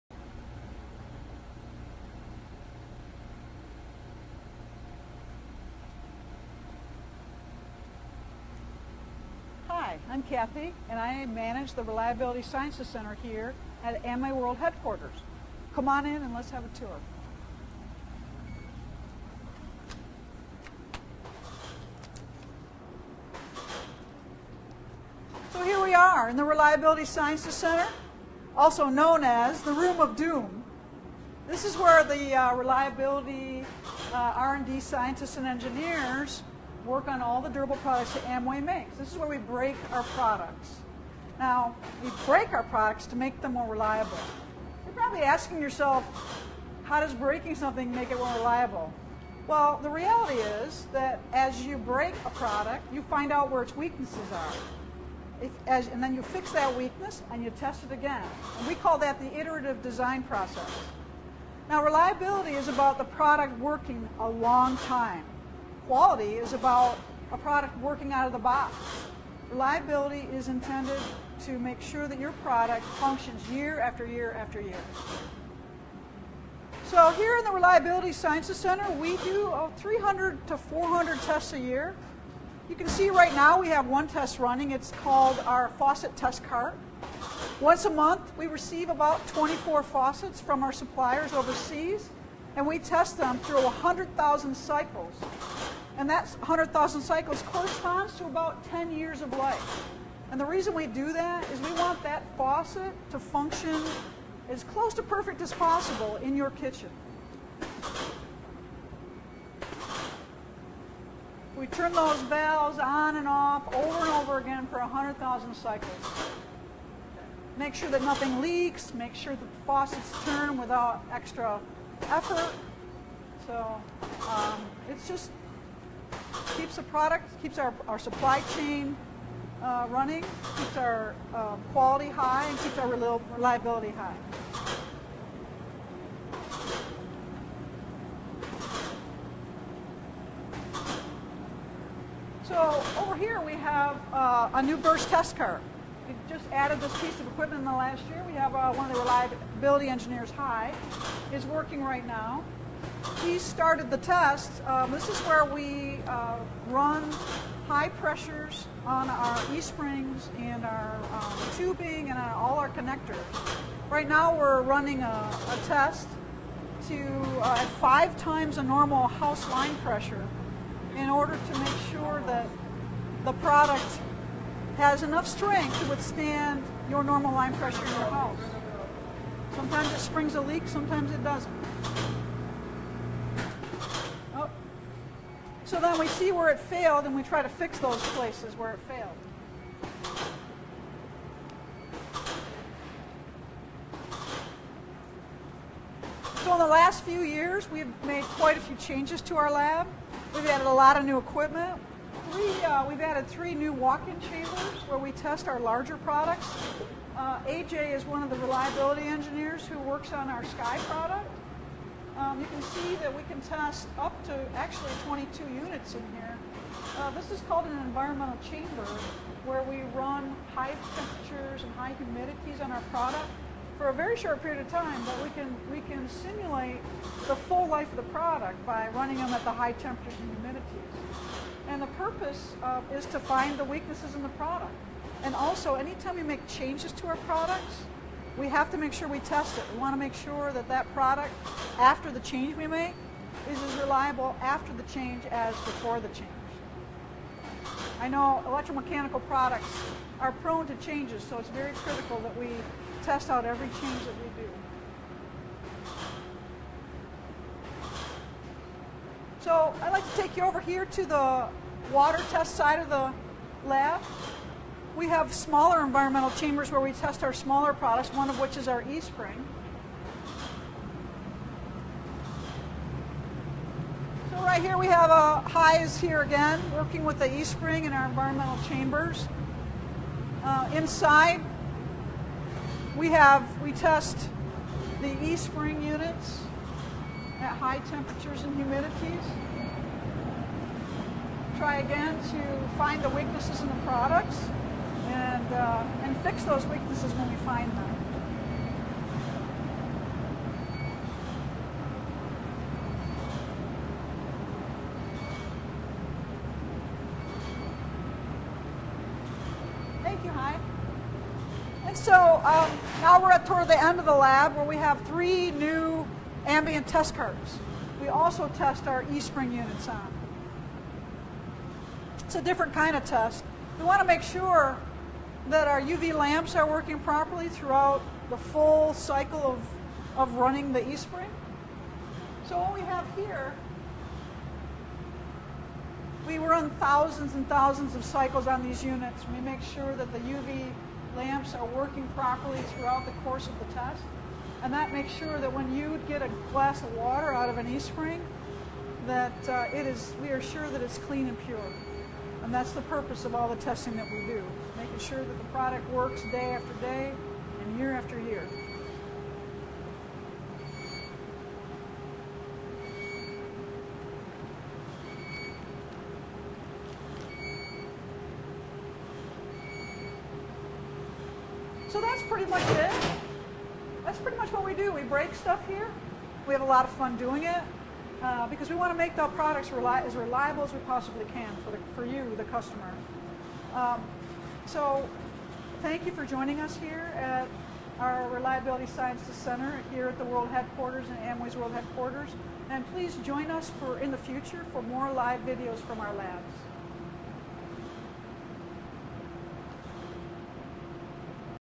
Amway Reliability Lab
Here is a live Facebook video from the Amway Reliability Lab in our World Headquarters, where our engineers are constantly breaking things to ensure the quality and reliability of our products!